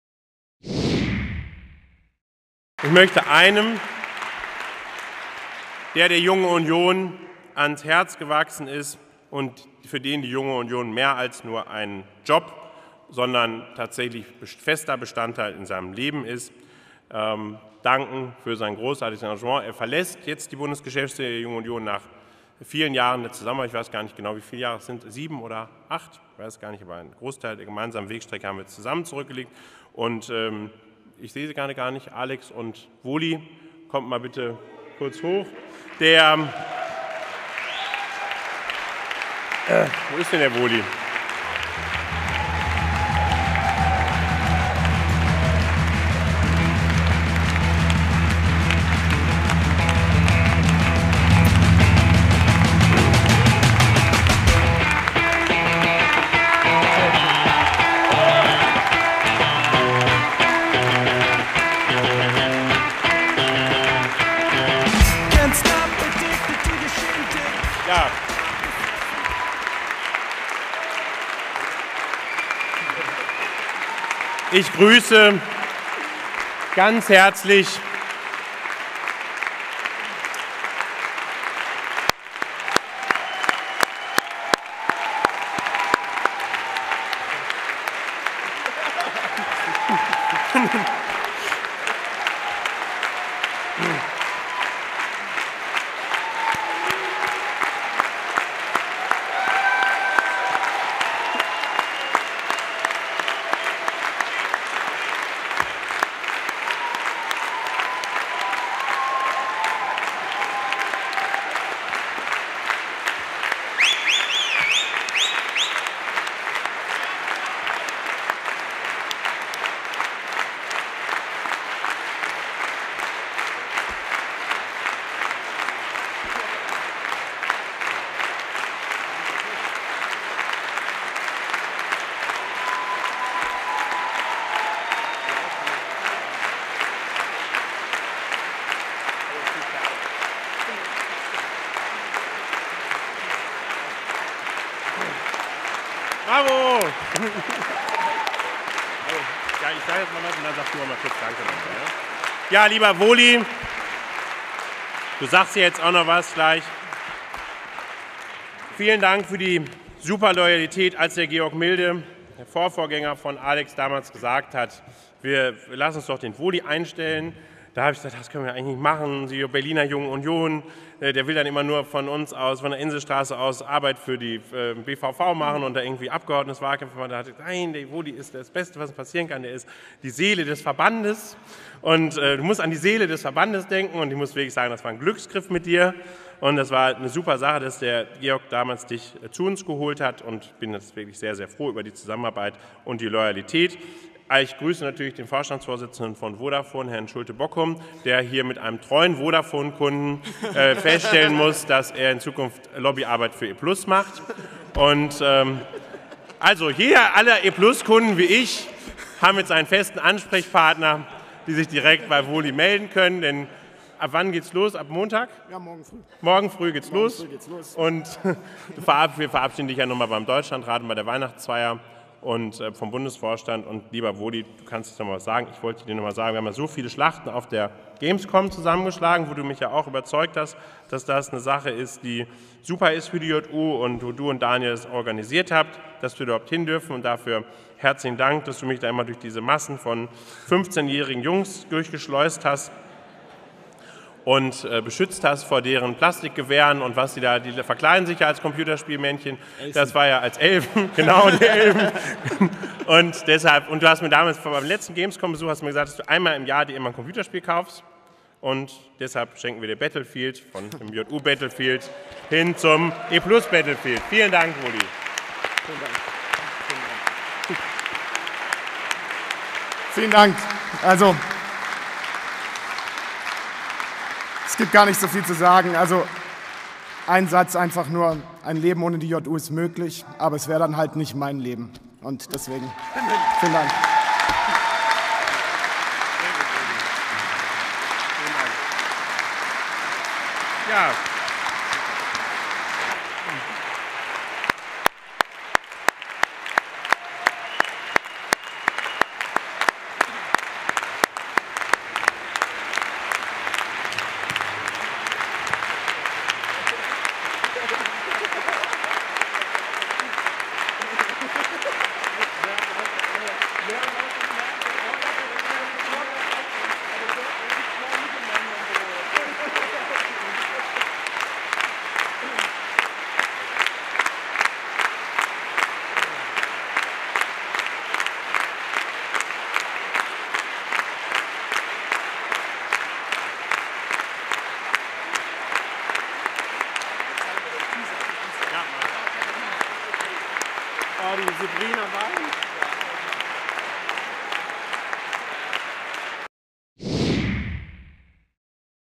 DLT13 +++ Verabschiedung von Christian "Wohli" Wohlrabe ++
Dieses Video habe ich von JUNGEUNIONTV gespiegelt:

"Nach drei Jahren im Dienst der JU Berlin und acht Jahren als Orga-Chef der JU Deutschlands hat Christian "Wohli" Wohlrabe im "fortgeschrittenen Alter" von 36 Jahren die JU verlassen. Stehende Ovationen begleiteten den sichtlich gerührten Wohli auf seinem Weg zum Podium. In seiner kurzen Ansprache brachte er seine Verbundenheit zur JU auf einen Nenner: "Ein Leben ohne die JU ist sicher möglich, aber es wäre dann halt nicht mein Leben". Wohli startete bereits einen Tag später bei seinem neuen Arbeitgeber, der E-Plus-Gruppe."